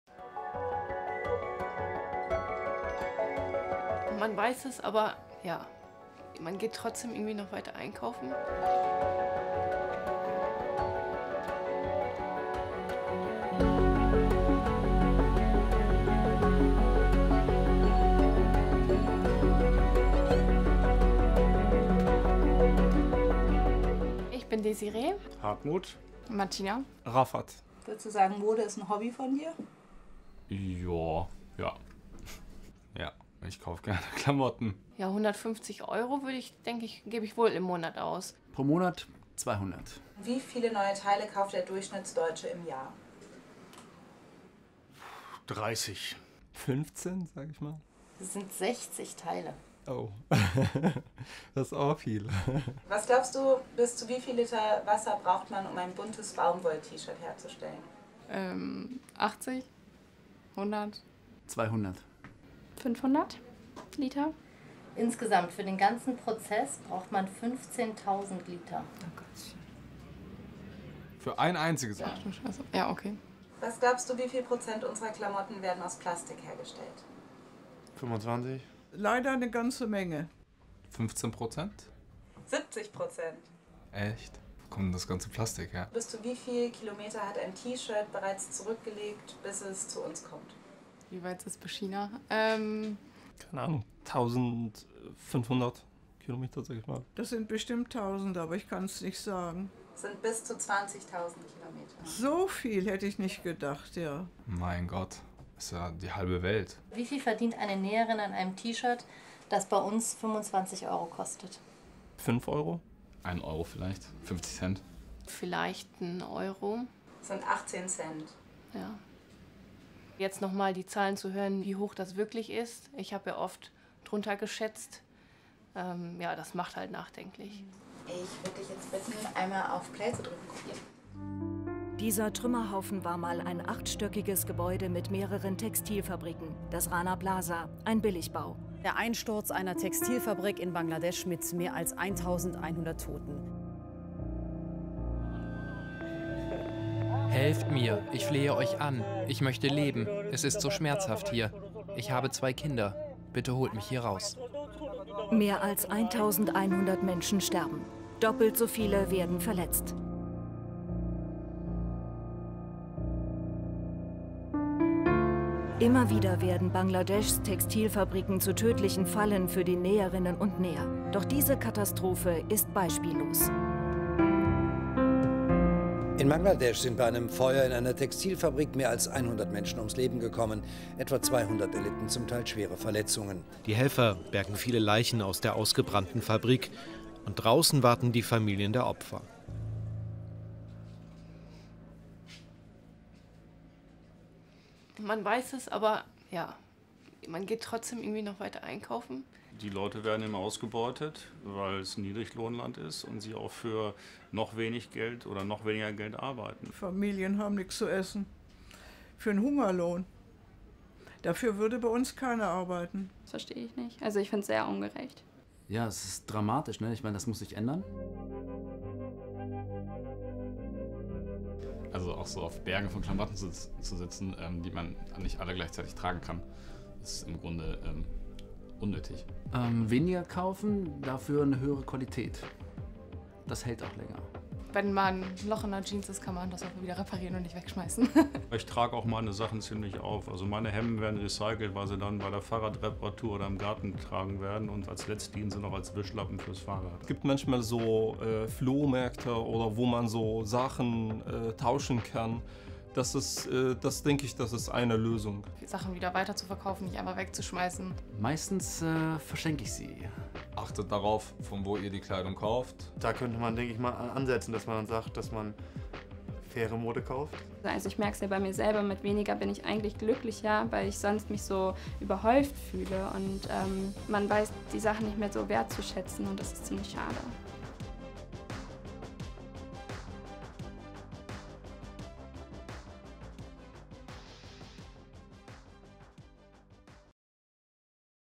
So ungerecht ist Fast Fashion – Was können wir ändern? Ein Experiment.
Schadet unser Konsumverhalten anderen? Darüber denkt man am liebsten gar nicht nach. Wir haben ein Soziales Experiment gewagt und zur Konfrontation mit dem eigenen Modekonsum eingeladen.